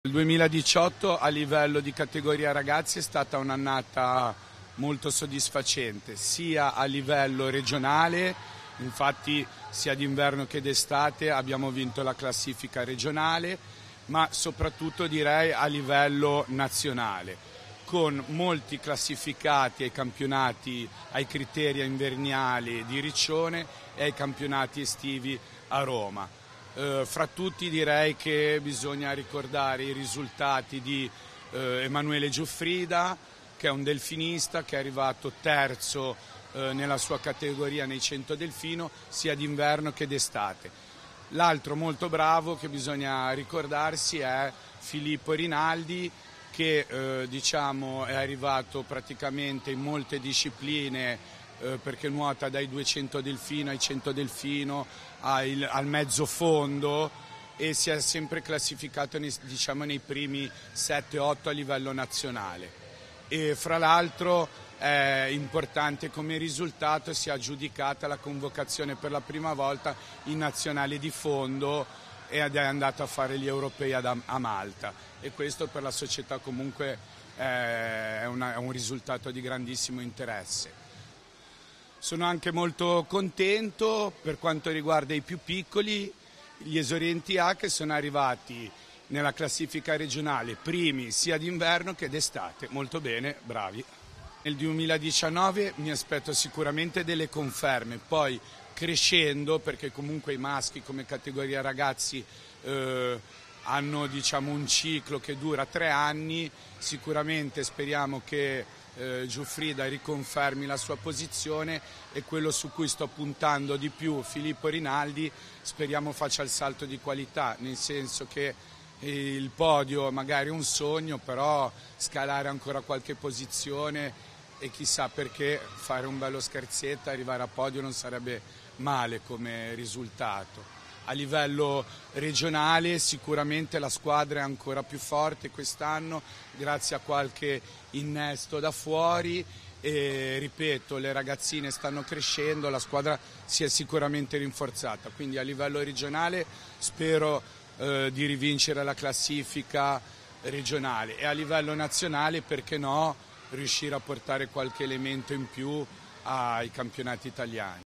Ragazzi ed Esordienti: la nuova linfa del Genova Nuoto
Nicola Morando, tecnico Genova Nuoto, ci parla delle prospettive di crescita del settore giovanile. Ecco i nomi dei giovani protagonisti delle attività della società della presidente Mara Sacchi.